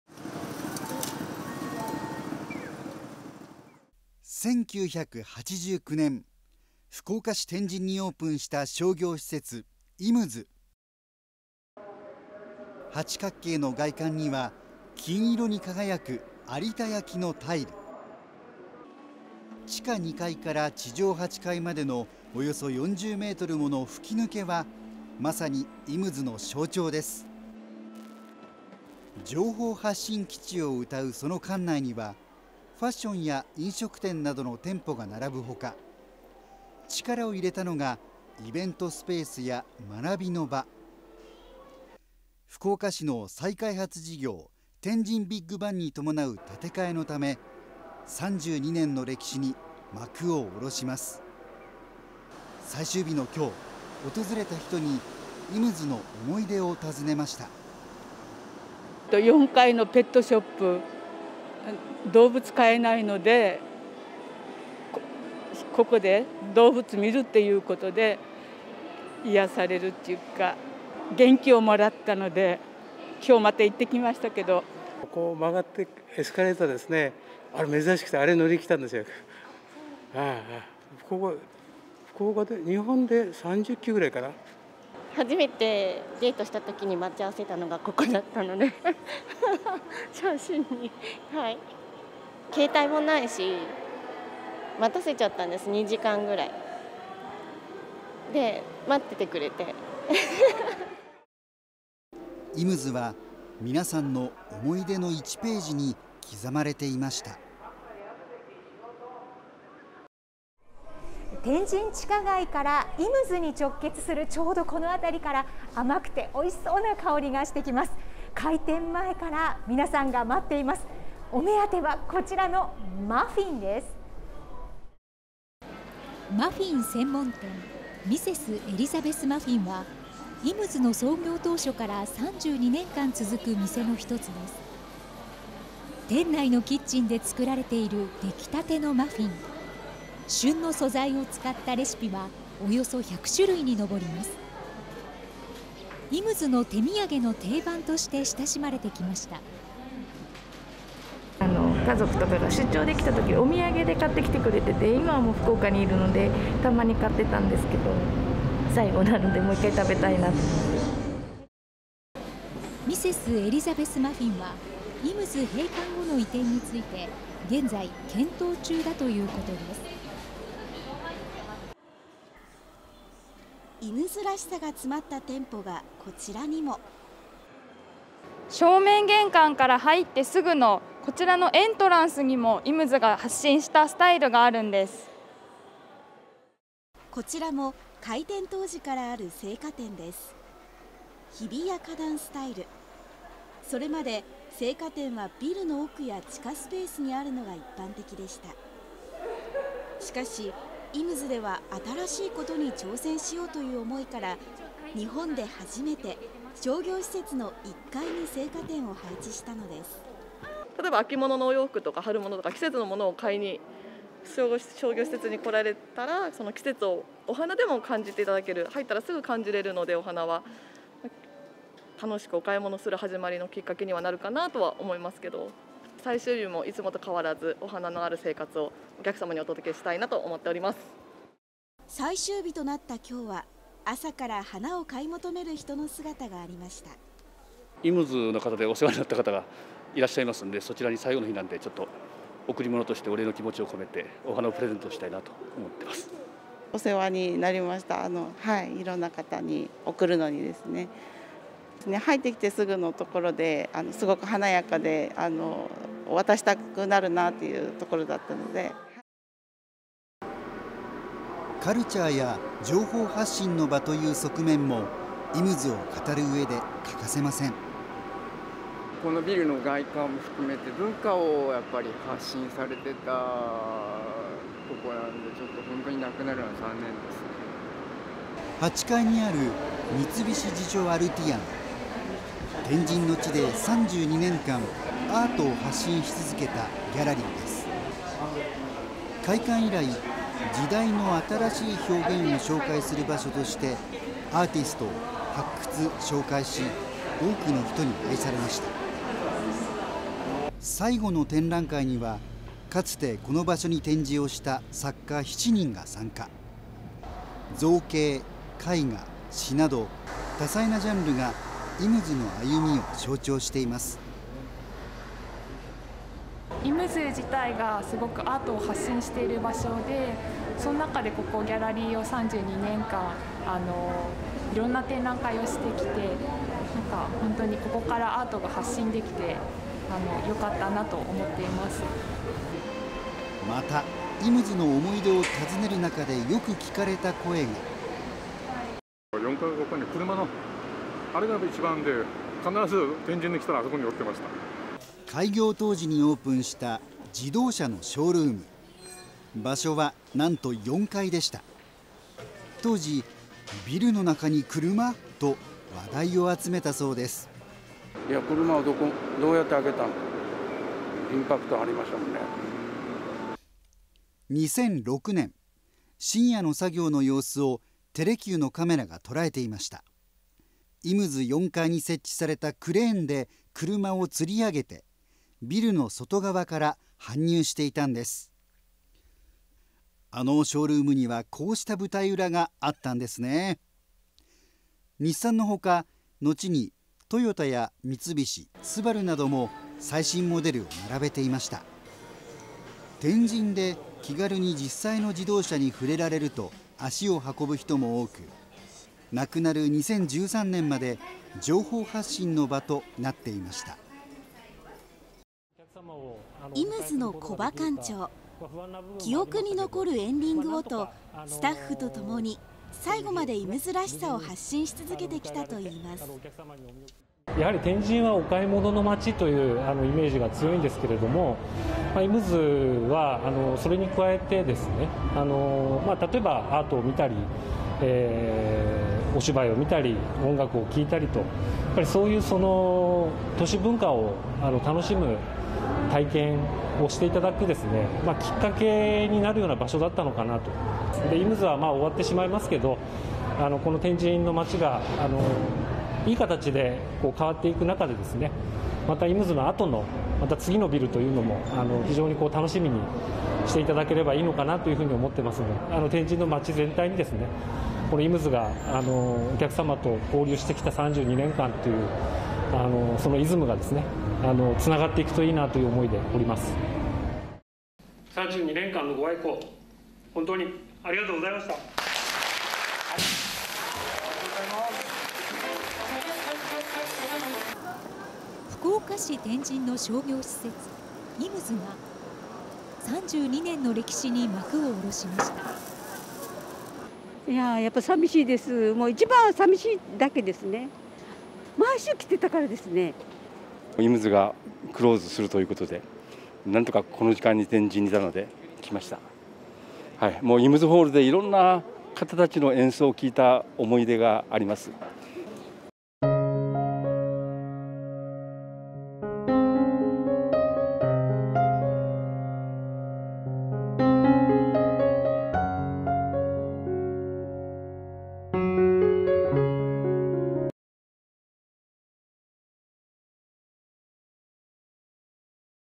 【あのトキ】情報発信基地 イムズ最後の営業（2021年8月31日放送）
【2021年8月31日放送】1989年に福岡市天神にオープンした商業施設「イムズ」は、八角形の外観と金色の有田焼タイル、地下2階から8階までの40メートルの吹き抜けが特徴でした。「情報発信基地」として、ファッションや飲食店の他、イベントスペースや学びの場にも力を入れていました。しかし、福岡市の再開発事業「天神ビックバン」に伴う建て替えのため、32年の歴史に幕を下ろします。最終日には多くの人が訪れ、思い出を語りました。イムズは多くの人々の思い出の場所として刻まれています。